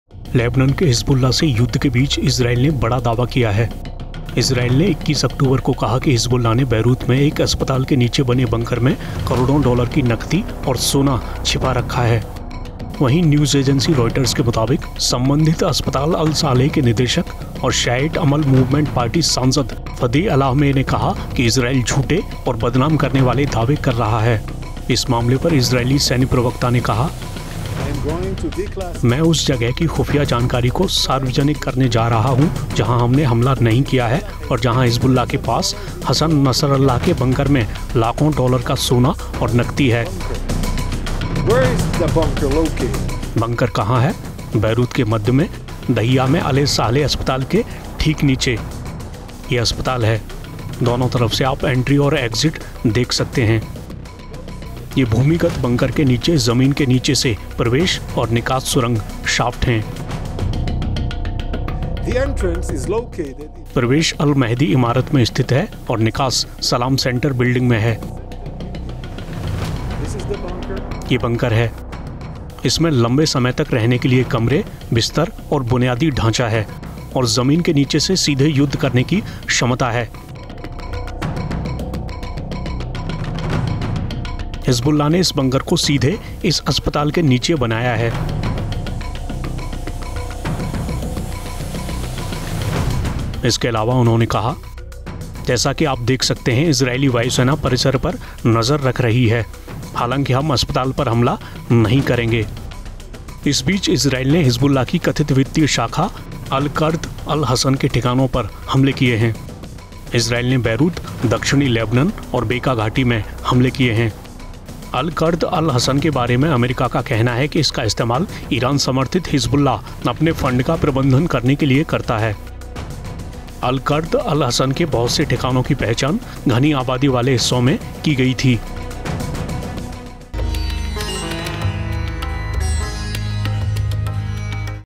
Israel Lebanon War: इजराइल का बड़ा दावा- 'Beirut के Hospital के नीचे है Hezbollah का खजाना' | N18G
लेबनन के हिजबुल्ला से युद्ध के बीच इजराइल ने बड़ा दावा किया है. इजराइल ने कहा है 21 अक्टूबर को कहा कि हिजबुल्ला ने बेरूत में एक अस्पताल के नीचे बने बंकर में करोड़ों डॉलर की नकदी और सोना छिपा रखा है. वहीं न्यूज एजेंसी रॉयटर्स के मुताबिक, संबंधित अस्पताल अल-साहेल के निदेशक और शियाइट अमल मूवमेंट पार्टी सांसद फदी अलामेह  ने कहा कि इजराइल झूठे और बदनाम करने वाले दावे कर रहा है.